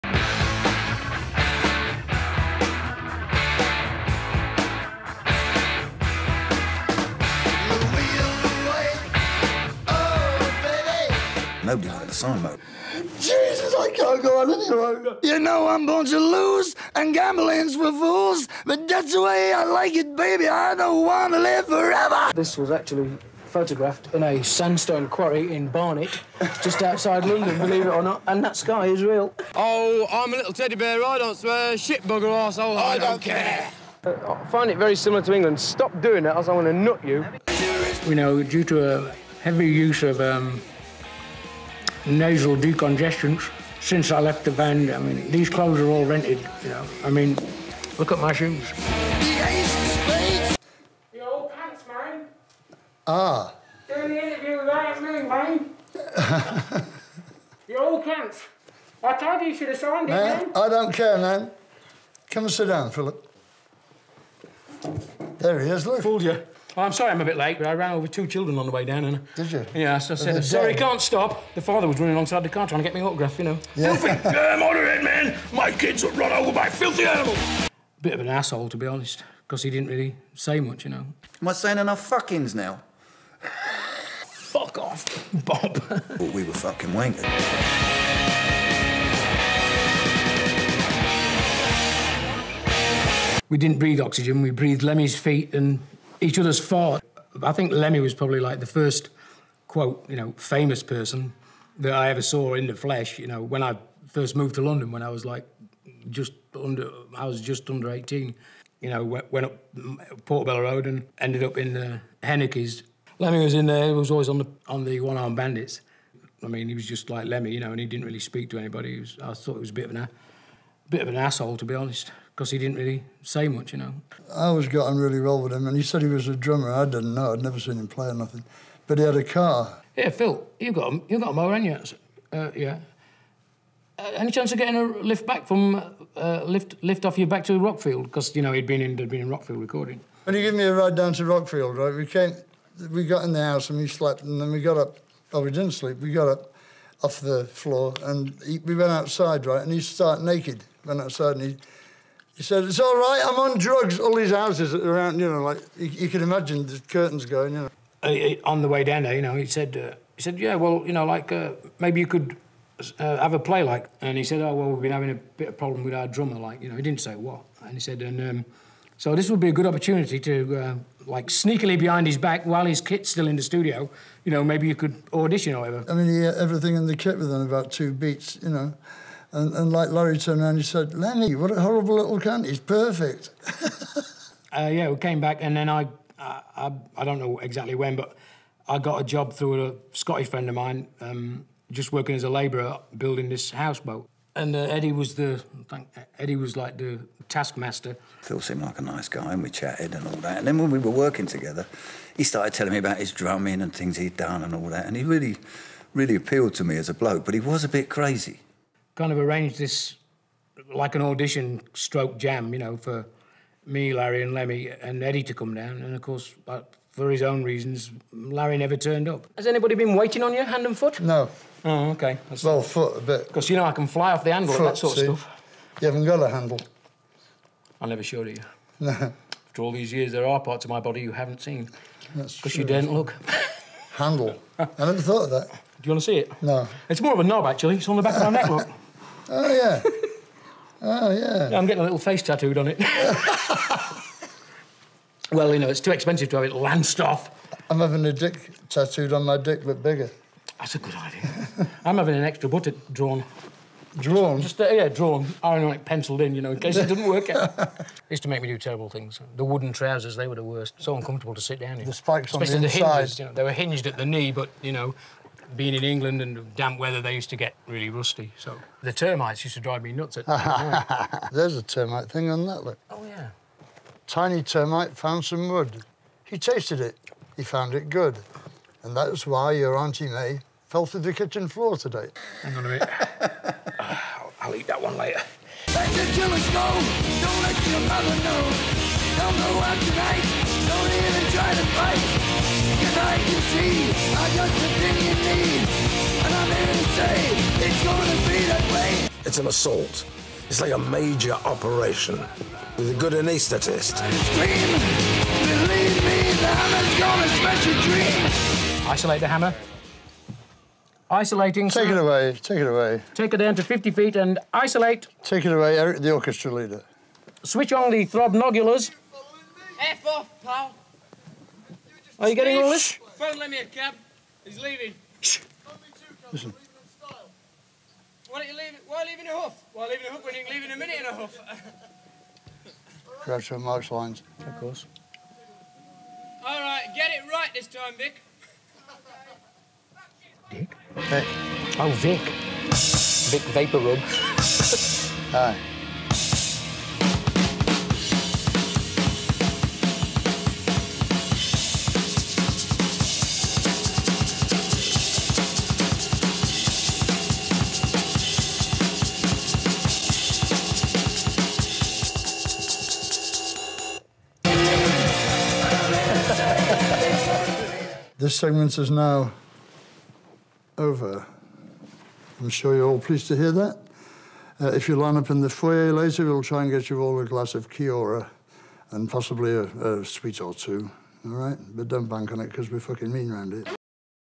The Best of Philthy Animal Taylor - Motörhead - (1)
Motörheads Philthy Animal is talking..!!...with Fast Eddie and Lemmy..!!

Some storys of the funniest the coolest and the best Drummer ever !!  (Part 1)